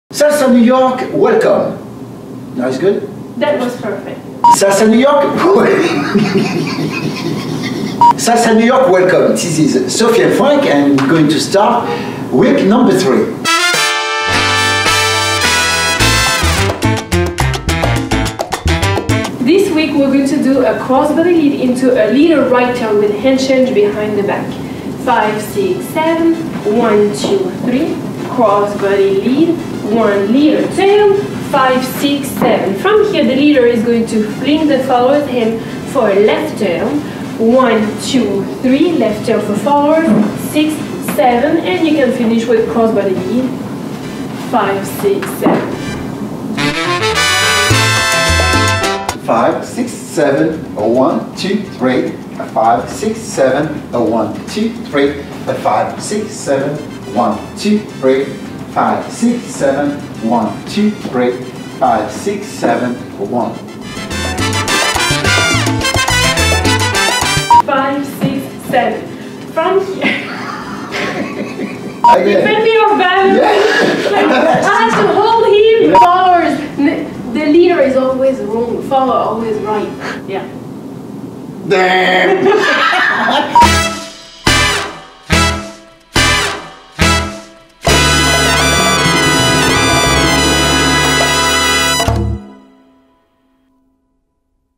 Leader Right Turn, Follower Left Turn: MOVE of the WEEK with Sophie & Franck!
Welcome to the MOVE of the WEEK with Sophie & Franck!

January 2019, week 2: Back break and basket!

Each week builds on the previous, and on week 4 we will review everything.

While some advanced dancers may be able to learn from these fast-paced clips, we encourage most dancers to ask an instructor for more detail before trying new moves on the social floor!

for events, classes, and more.
Find Life is Now on Amazon and support the dance community